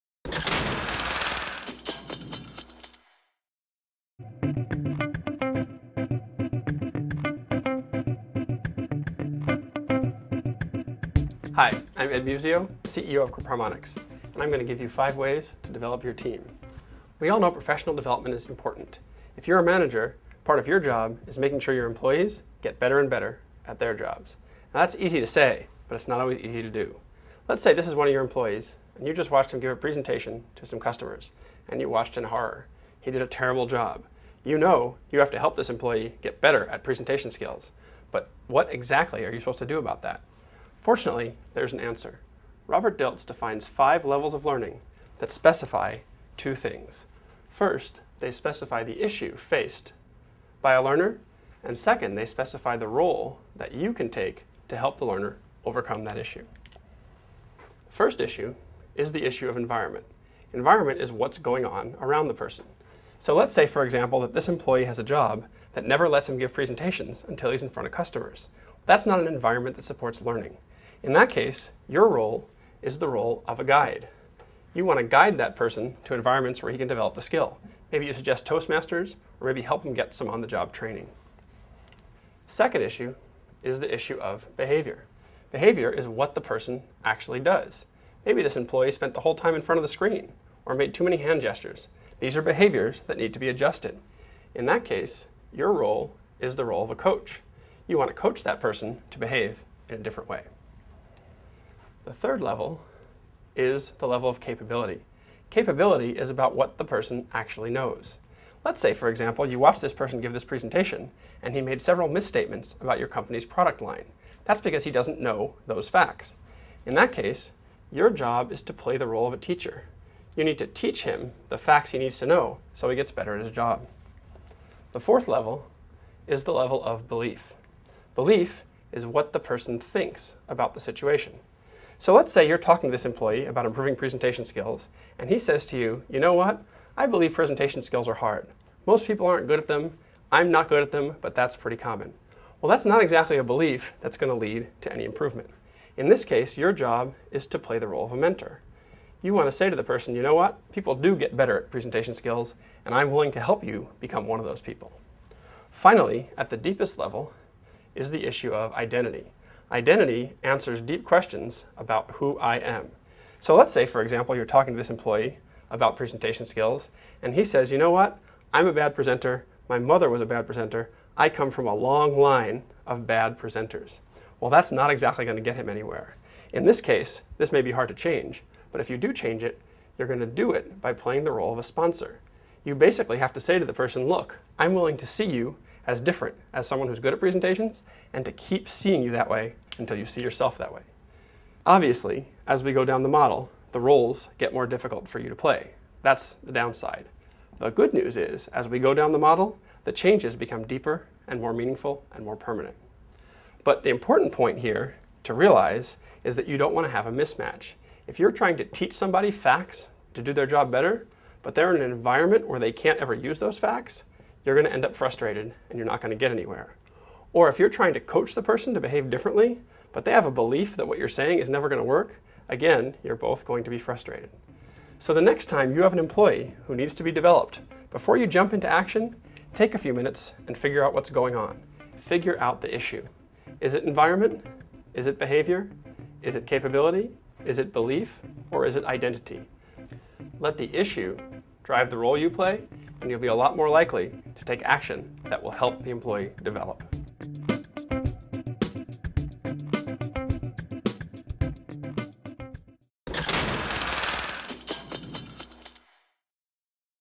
Five Ways to Develop Your Team
- Ed Muzio, CEO of Group Harmonics and author of "Make Work Great," describes Robert Dilts' levels of learning as they apply to the manager's role in employee development. Ed is also author of "Four Secrets to Liking Your Work."  Dilts is an expert in NLP (Neuro Linguistic Programming) and his levels of learning model applies to many aspects of workplace development, including skill building, behavior change, and the awareness of the impact of deeply held beliefs.